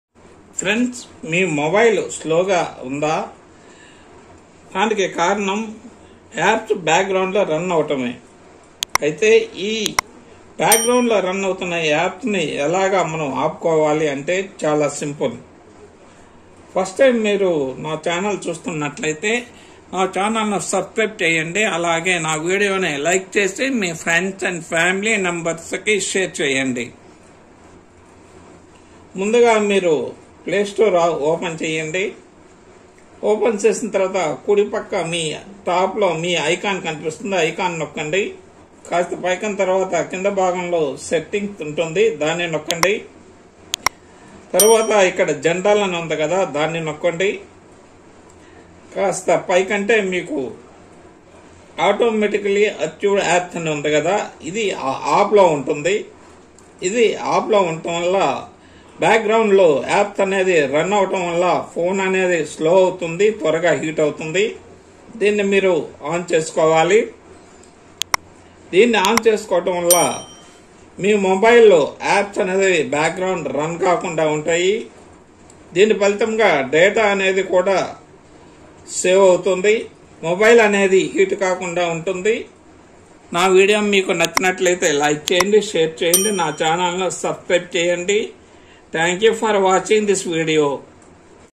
మీ ఫోన్ ఫాస్ట్ గా రన్ కావాలంటే ఫ్లే స్టోర్ లో ఈ సెట్టింగ్స్ చేసుకోవాలి. BY PVSNAGP TECH CHANNEL.
మీ ఫోన్ ఫాస్ట్ గా రన్ కావాలంటే ఫ్లే స్టోర్ లో ఈ సెట్టింగ్స్ చేసుకోవాలి.